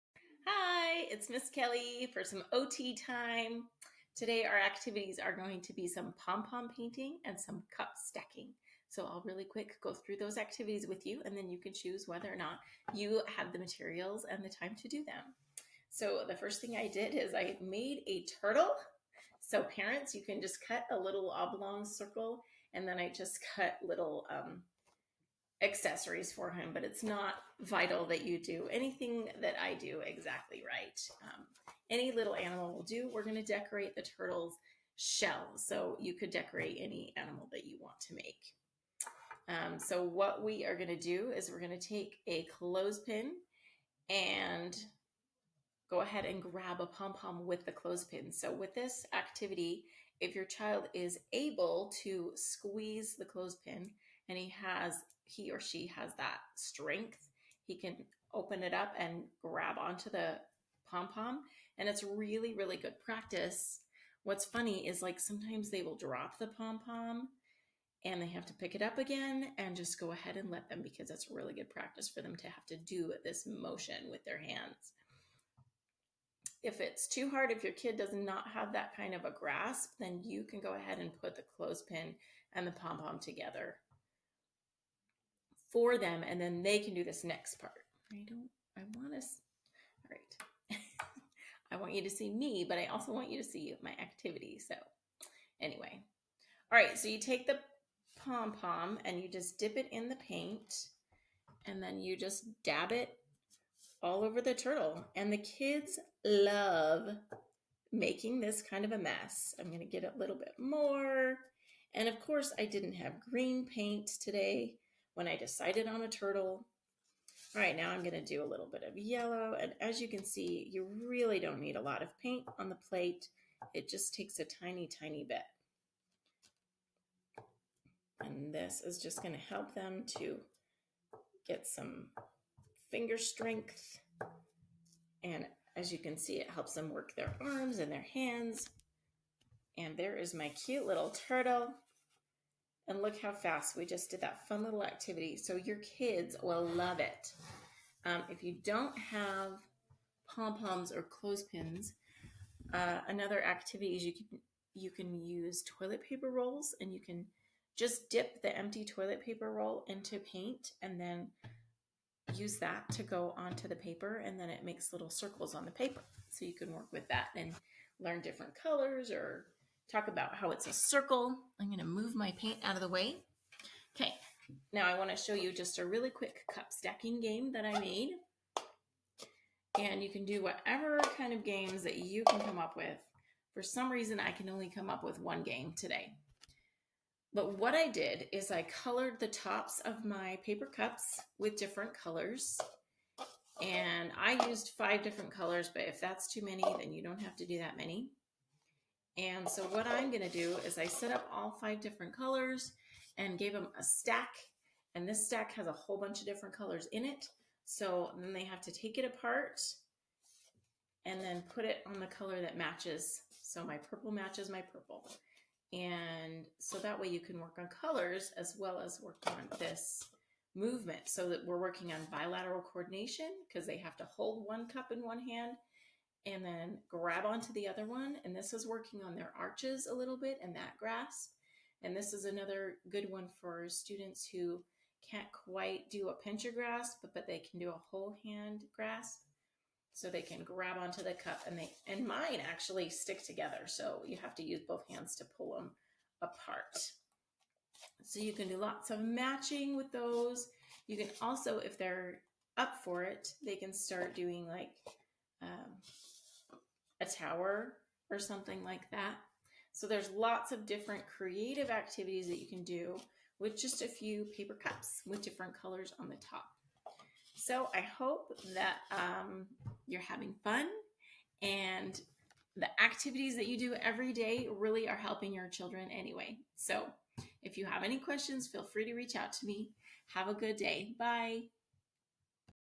Pompom painting and cup stacking
Fine motor activities where kiddos can use pompoms and clothes pins to paint a turtle shell. They can also work on hand arch movements by doing a cup stacking game. Appropriate for preschool age or developmentally delayed children.